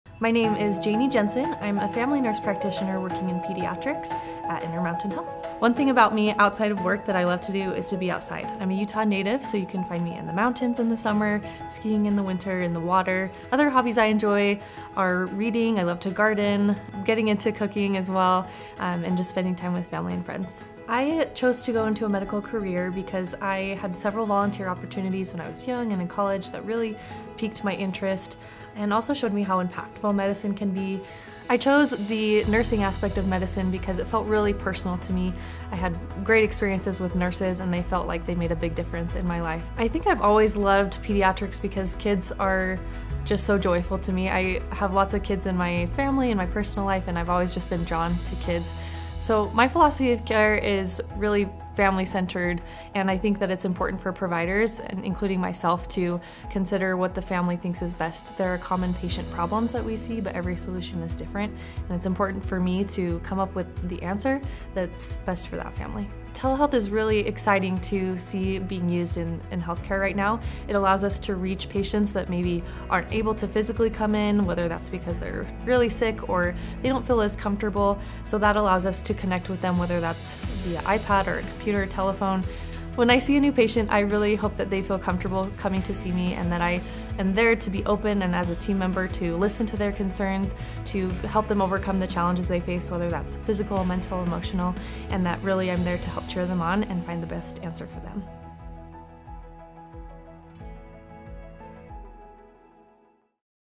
Janie Jensen, NP | Intermountain Health
Meet Janie Jensen, a dedicated pediatric nurse practitioner passionate about supporting children and their families throughout their wellness journey. Janie is committed to fostering an environment where patients feel comfortable and open, allowing her to provide the guidance and emotional support needed to navigate both physical and mental health challenges. She values keeping families involved in the care process, understanding that every child is unique and requires personalized solutions to help them achieve their healthiest lives possible.